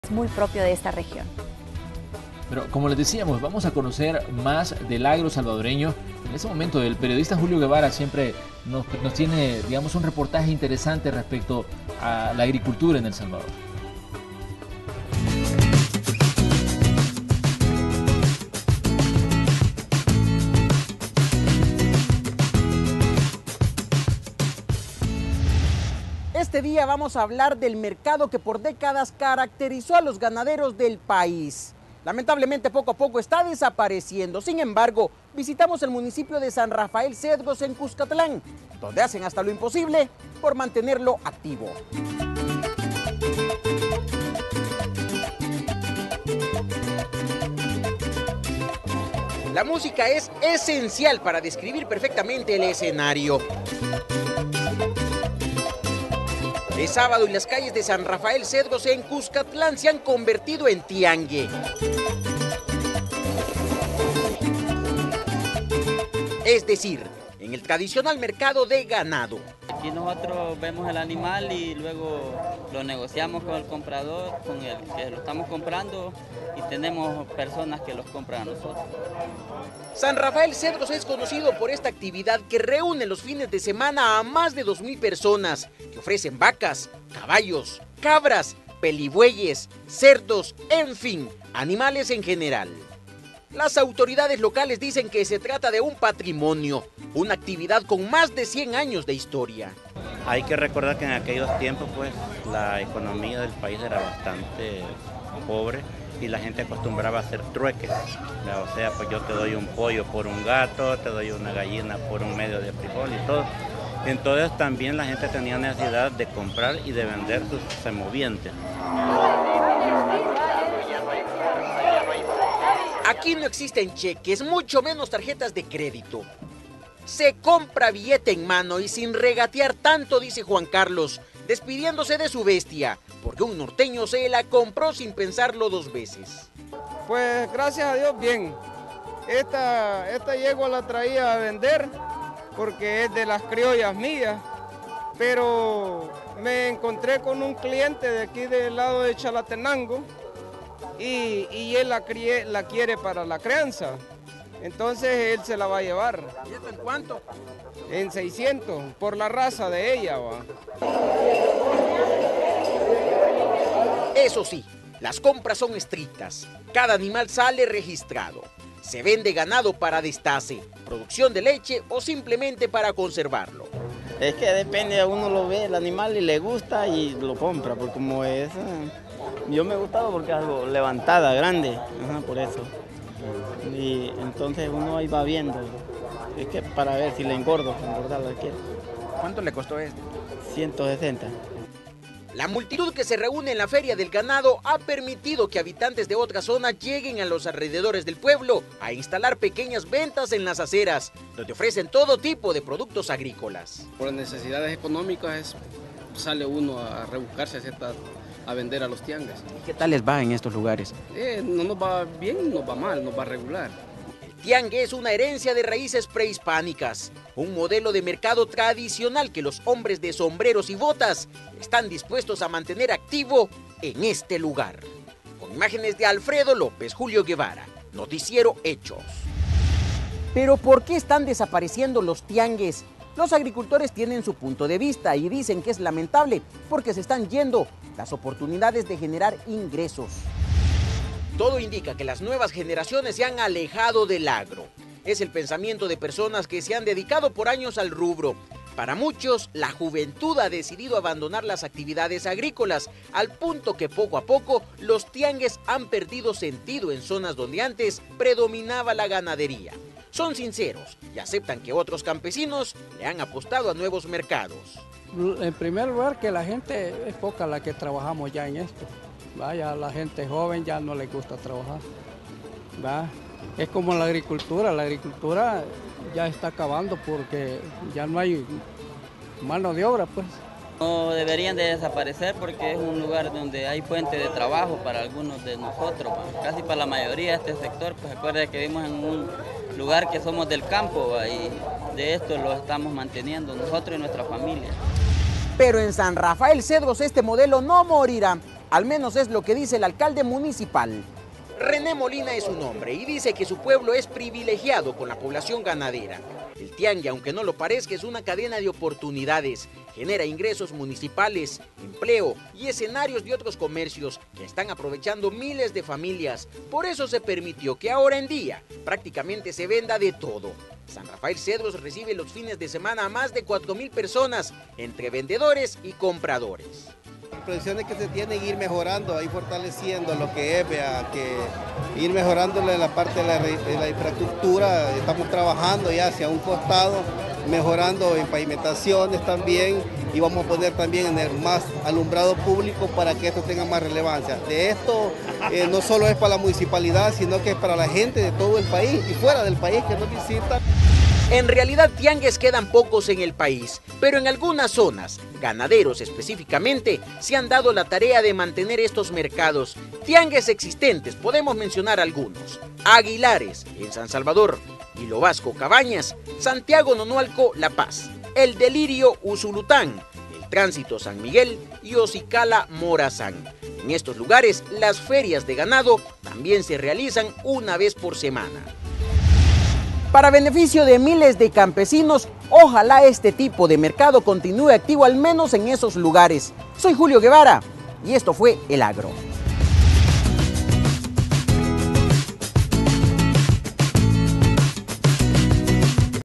Los "tiangues" se niegan a desaparecer
En el segmento de el "El agro" le mostramos cómo se llevan a cabo los mercados ganaderos o también conocidos tradicionalmente como "tiangues", los que a pesar del paso de los años se niegan a desaparecer.